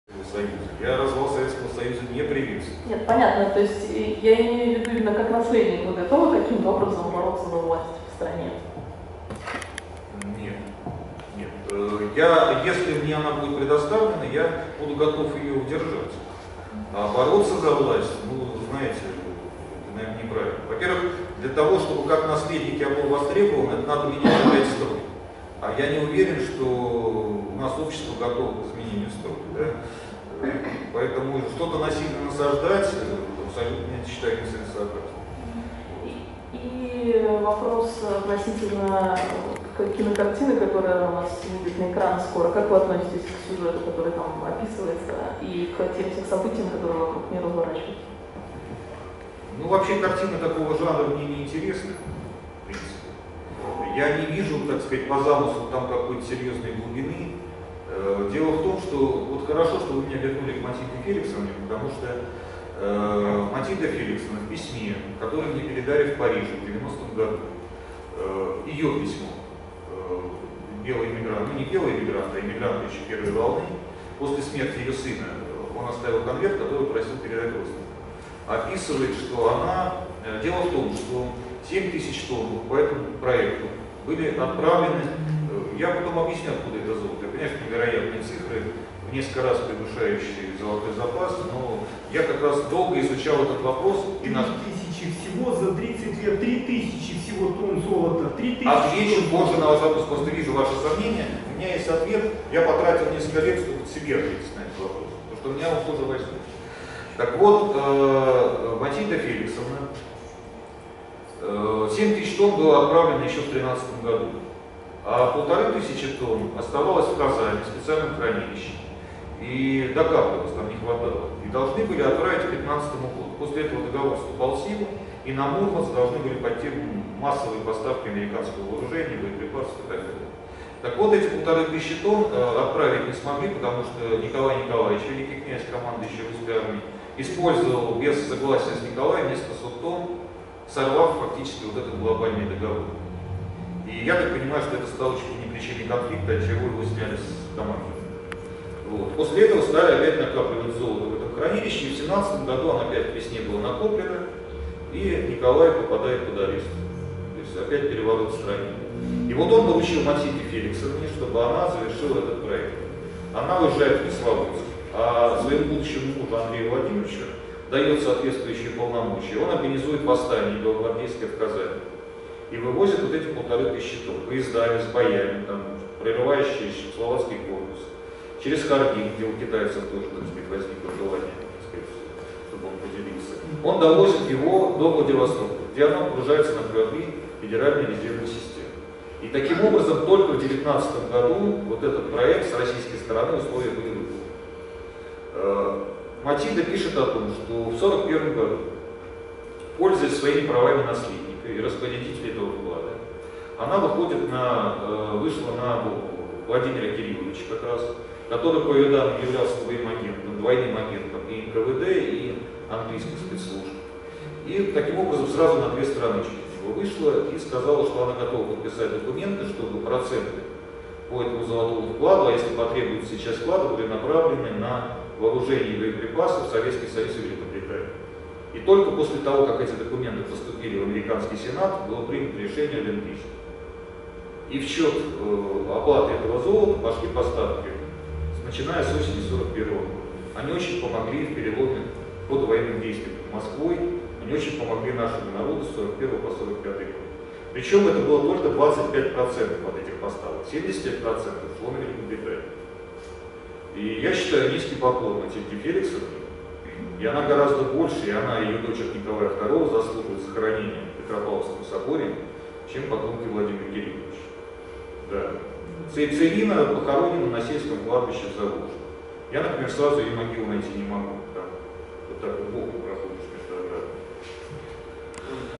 Ответы на вопросы
Пресс-конференция Константина Севенарда в "Интерфаксе" 14 сентября . Ответы на вопросы о борьбе за власть, о фильме "Матильда" и о тоннах золота